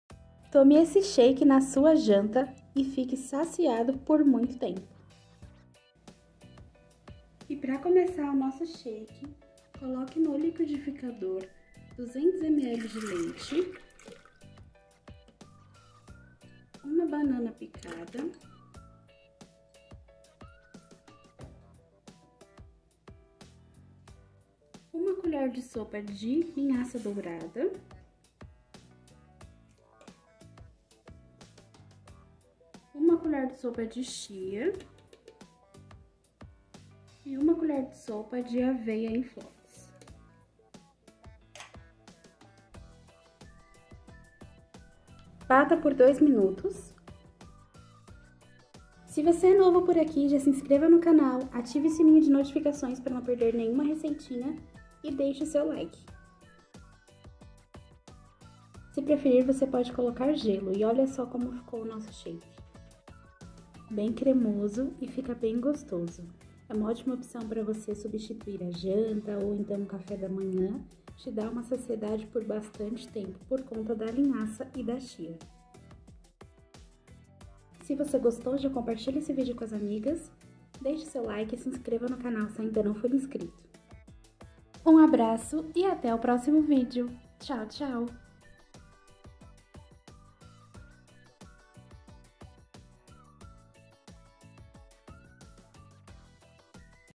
Tome Esse Shake na Sua Janta | Shake Emagrecedor
Olá Sejam bem vindos (as) ao canal Amor & Sabor!!!

Hoje vamos de shake de banana, muito bom para substituir alguma refeição do dia, essa bebida dá saciedade por conter a chia e a linhaça, além de ser bem saudável.

❤️INGREDIENTES:

200 ml de leite integral ou dê sua preferência
1 banana                                                                                                                                                                     1 colher de sopa de linhaça
1 colher de sopa de chia
1 colher de sopa de aveia

Acompanhe nossas receitas: videos toda terça, quinta e sábado às 18:20hs